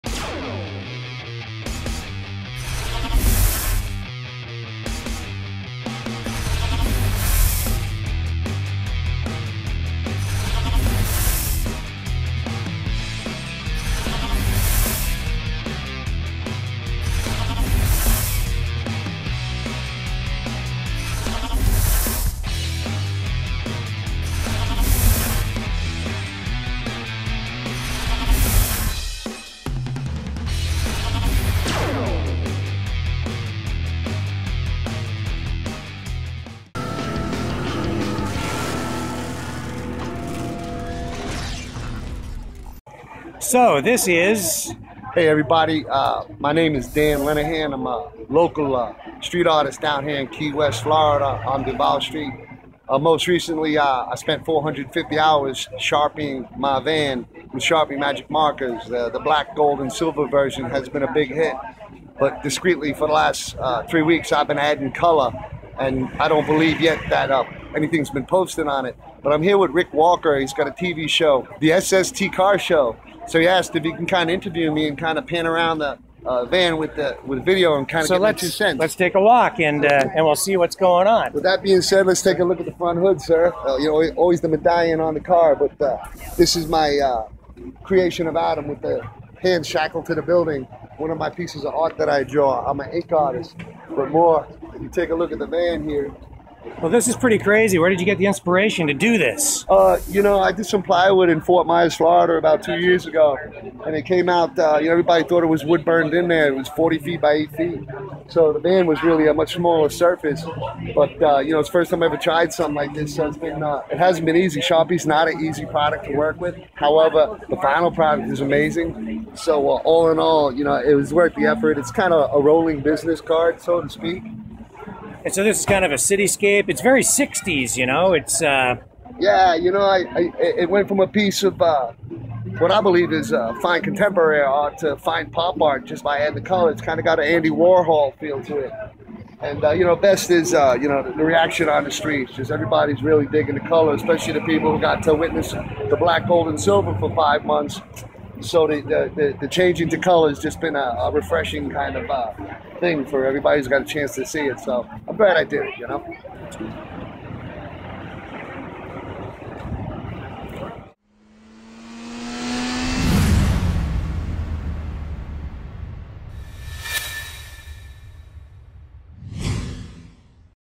Van turned into Rolling Work of Art
100 sharpies in total were used on this van to give it its unique look. Rick Walker, host of the SST Car show stumbled upon this unique minivan along the streets of Key West, Florida.